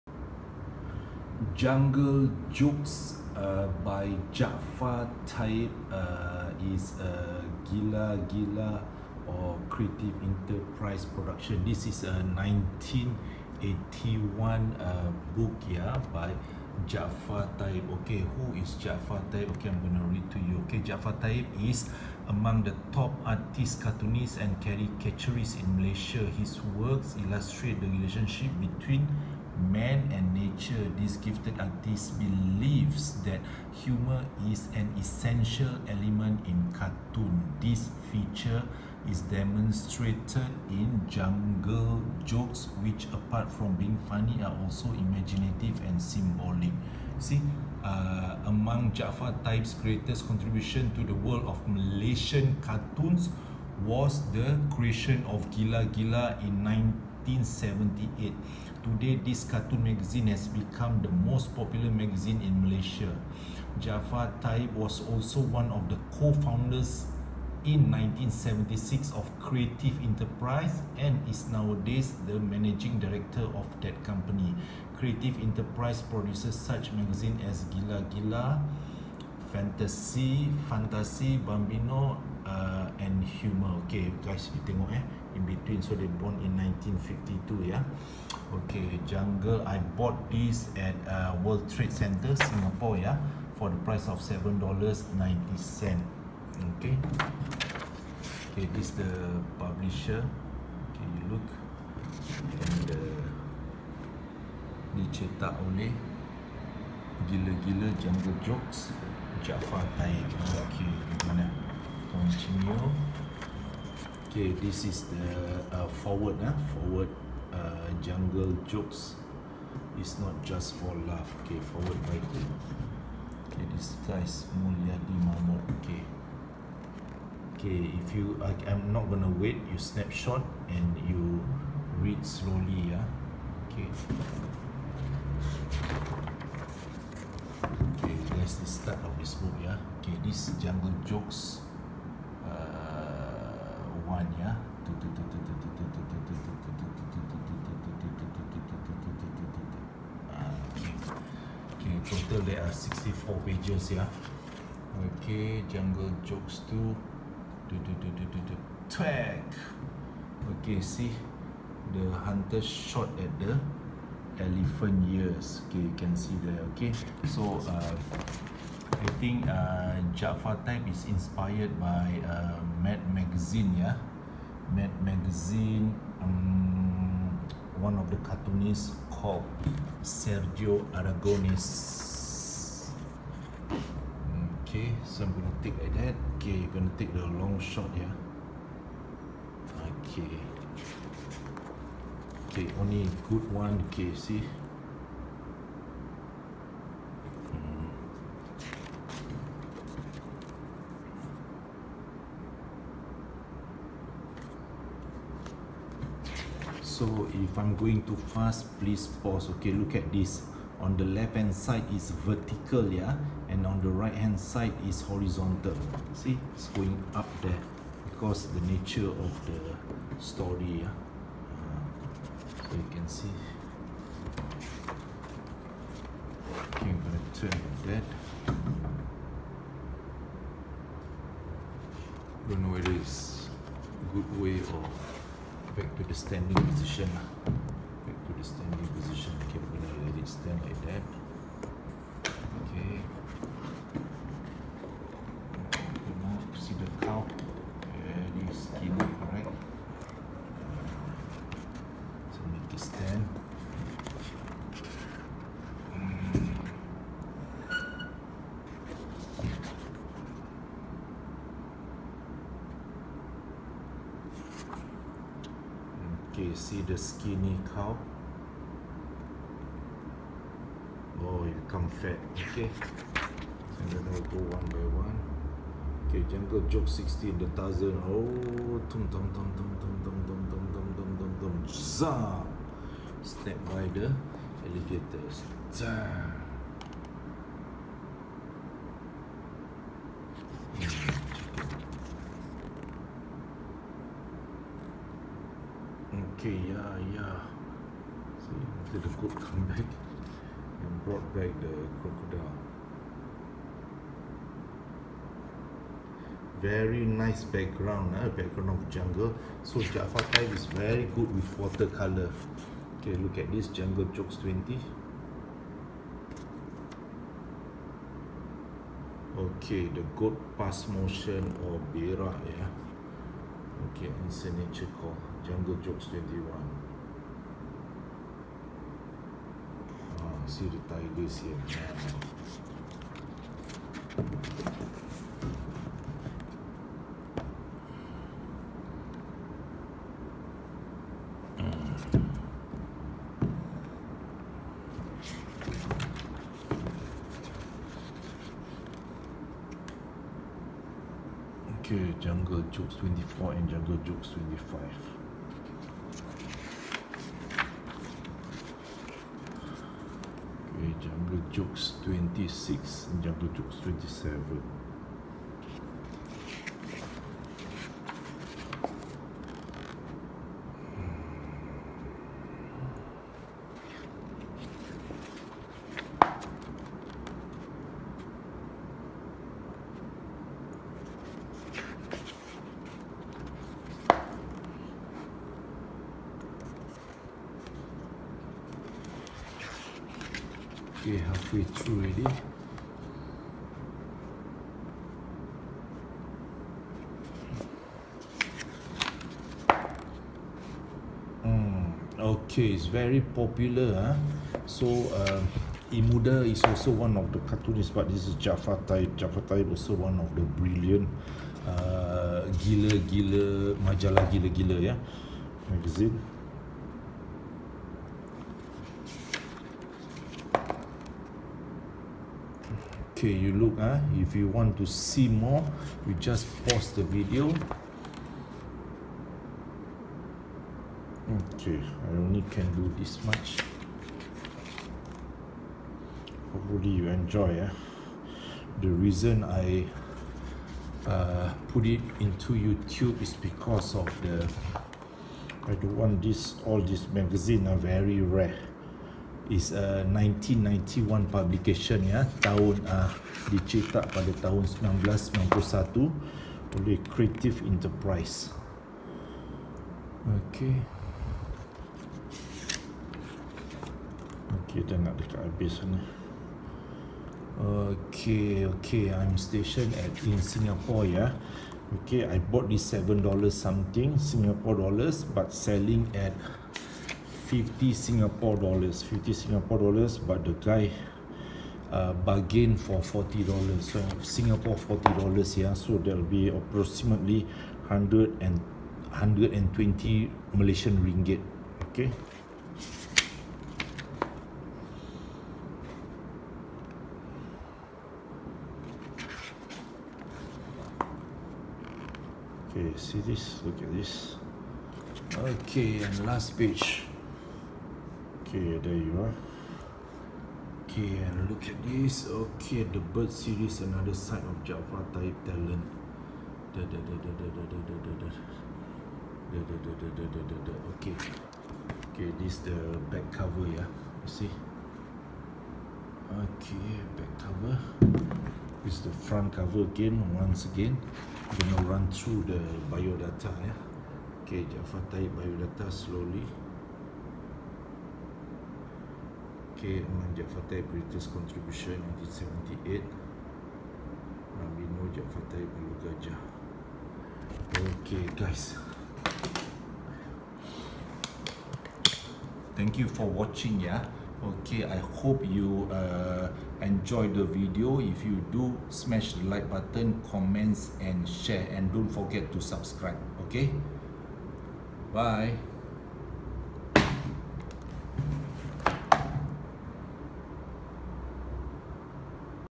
Jaafar Taib - Jungle Jokes (GILA-GILA)
Jaafar Taib is among the top artists and cartoonists in Malaysia. He is one of the co-founders of GILA-GILA magazines in 1978. Jaafar Taib was born in Alor Gajah, Melaka (Malaysia) on July 28, 1952.